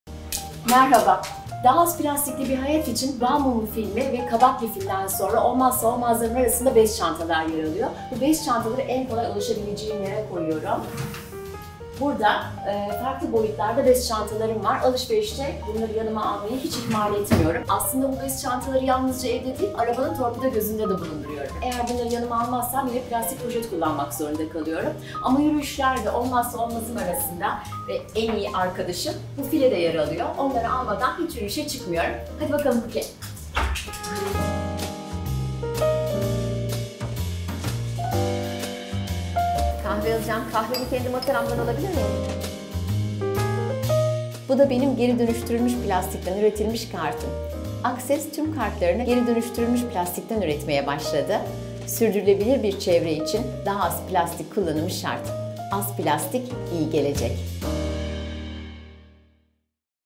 Bez Torbam ve Köpeğim Cookie Olmadan Asla Dışarı Çıkmam! :) - Çevre İçin Küçük Dev Adımlar
Daha az plastikli bir hayat için, olmazsa olmazlarım arasında bez çantalar yer alıyor.
Bu bez çantaları evde en kolay erişebileceğim yerlere koyuyorum.  Arabamda torpido gözünden de eksik etmiyorum.

Az plastik, iyi gelecek...